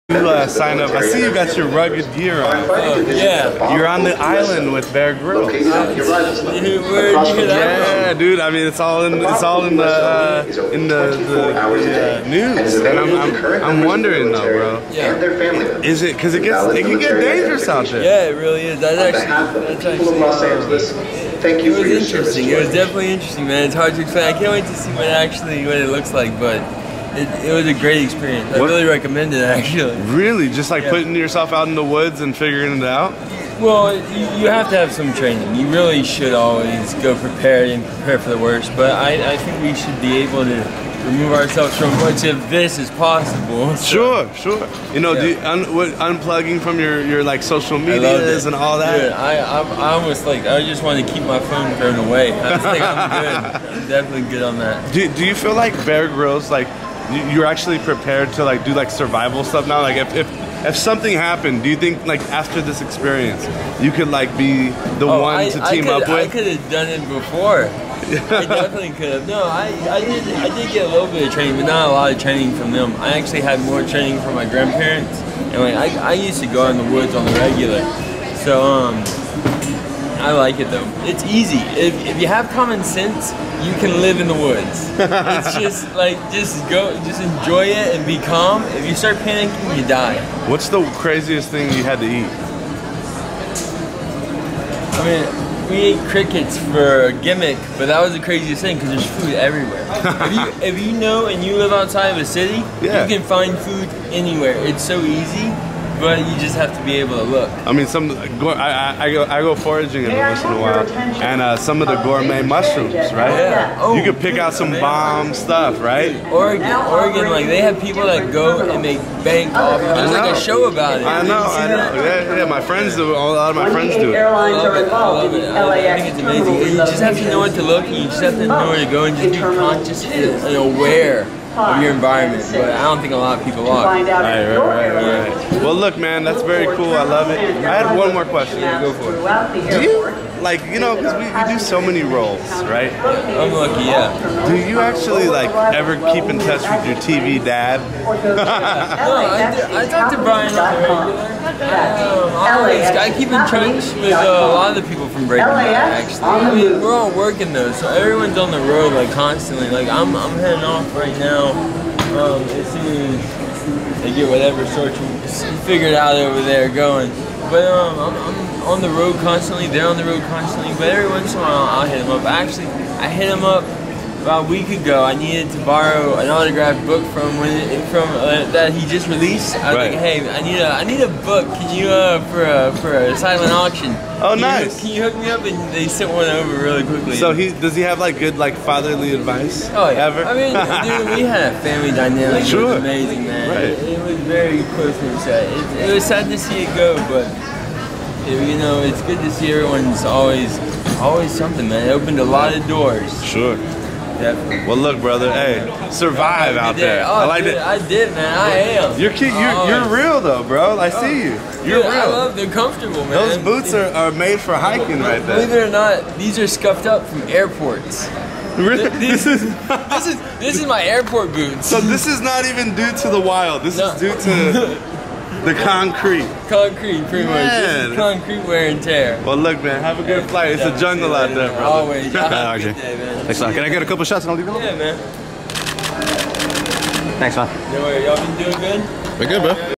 Breaking Bad Star RJ Mitte Chatting About Filming With Bear Grylls
Thursday, May 18, 2017: RJ Mitte was spotted at LAX, talking about his appearance on the Bear Grylls show, The Island.  Stating that the craziest thing he had to eat was crickets, for a joke, and that the key to survival in the wild is being aware of your surroundings.  X17online.com